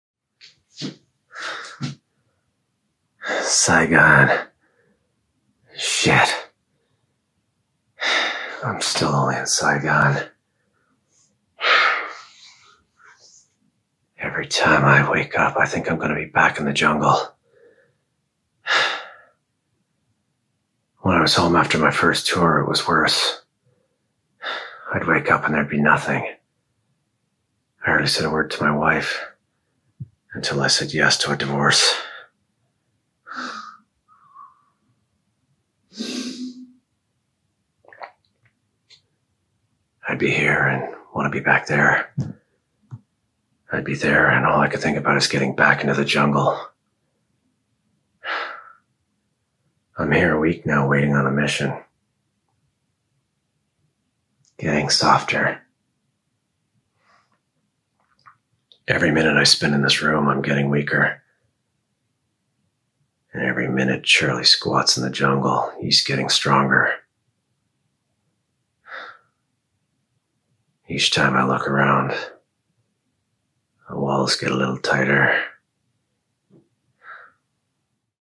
Byron Brydges - Apocalypse Now by Francis Ford Coppola - Monologue Slam Online - August 20, 2020
Byron Brydges
Monologue Slam 5.0 entry.
Apocalypse Now by Francis Ford Coppola.
A  masterpiece of the New Hollywood era...